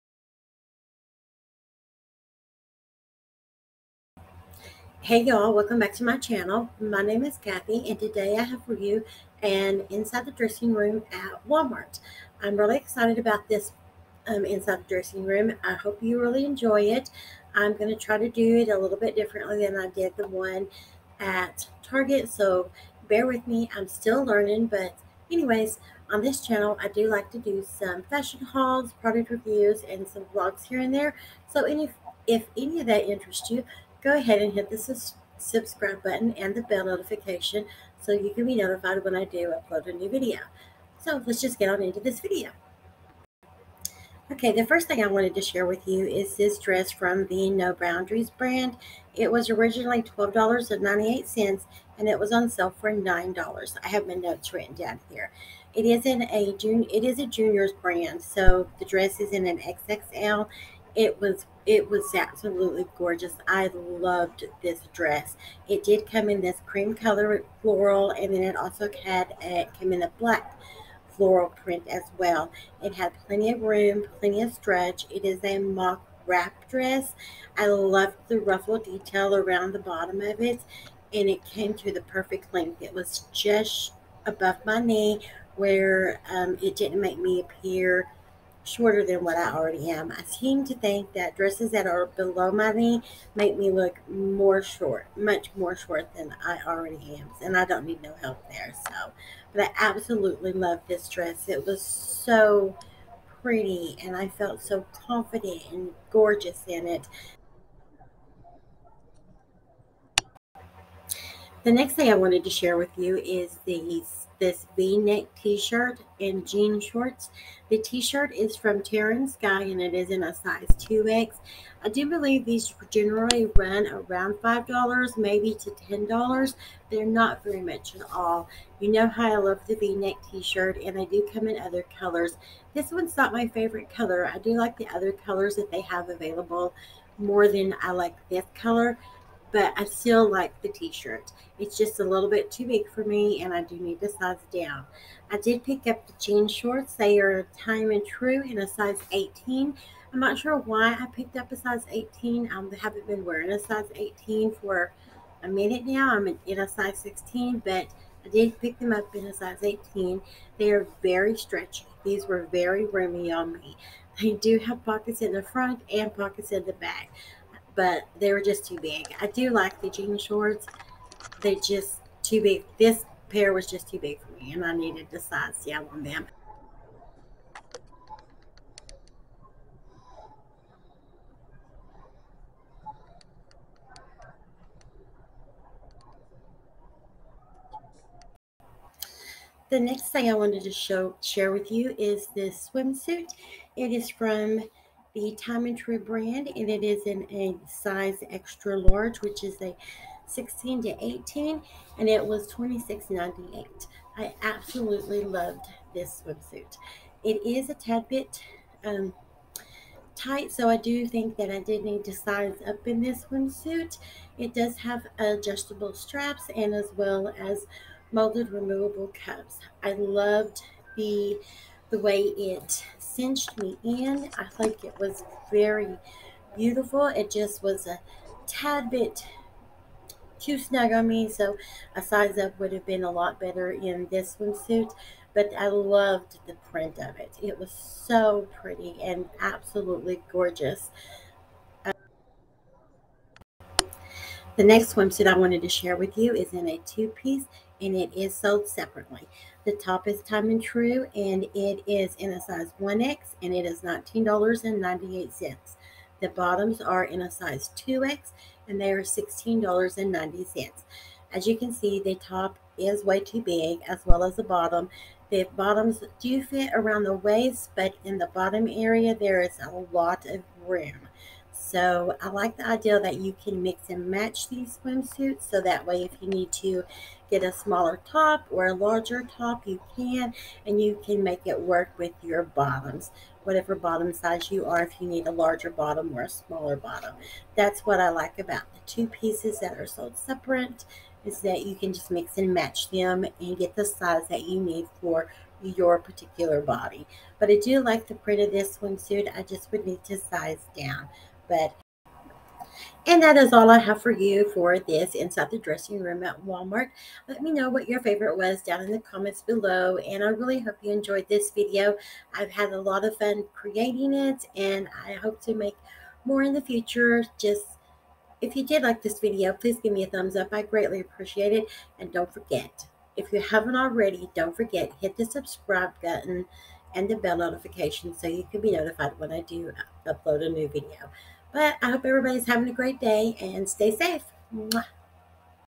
Swimsuits, dress, tee, shorts Inside the Dressing Room @ Walmart
210 pounds
Bra size 38C
waist 44 inches
hips 46 inches

I had so much fun creating this Inside the dressing room at Walmart.  They had a great selection of plus size fashion.